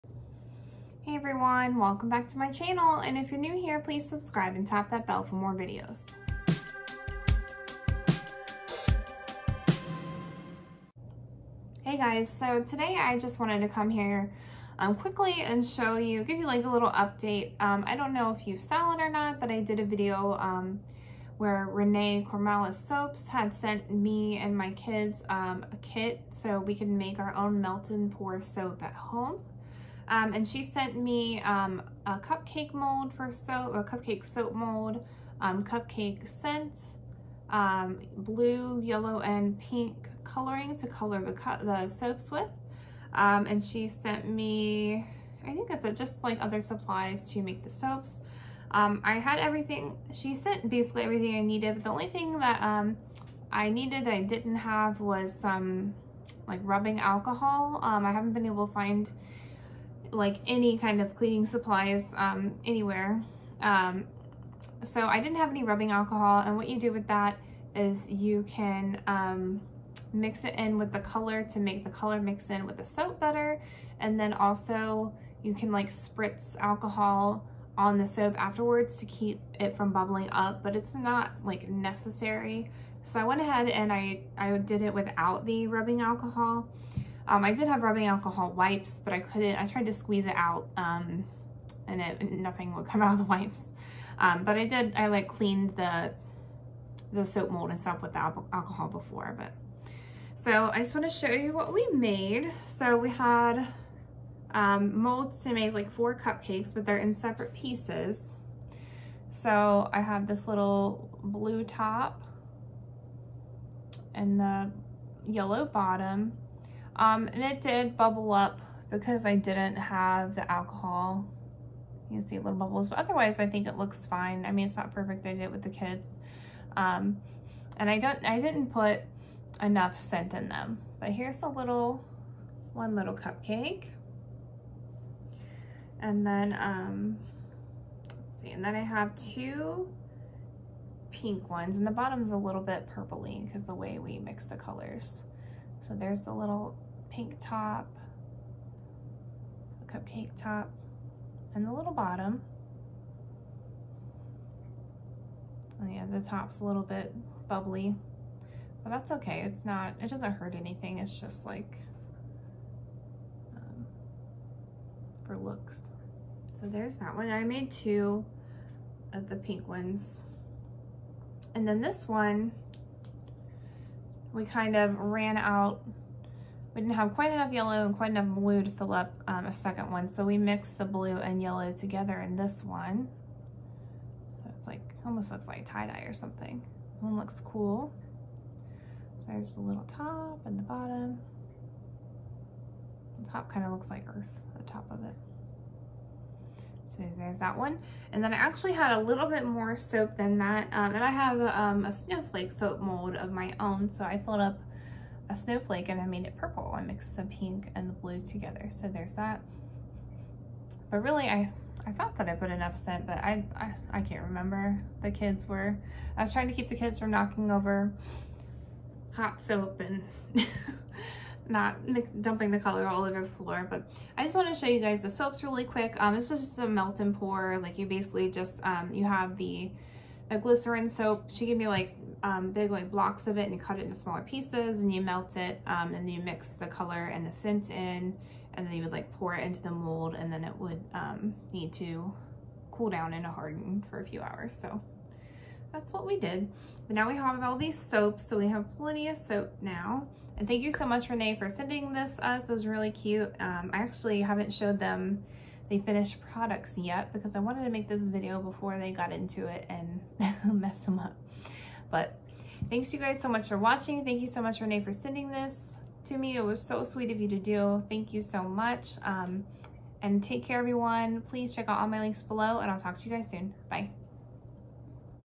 We made Soap! | Soap Share | Melt and Pour Soap
Hello! My sweet friend at Renee Cormalis Soaps sent my kids and I a nice soap kit. See how it turns out in the video. Thanks for stopping by. Enjoy!

My Etsy store: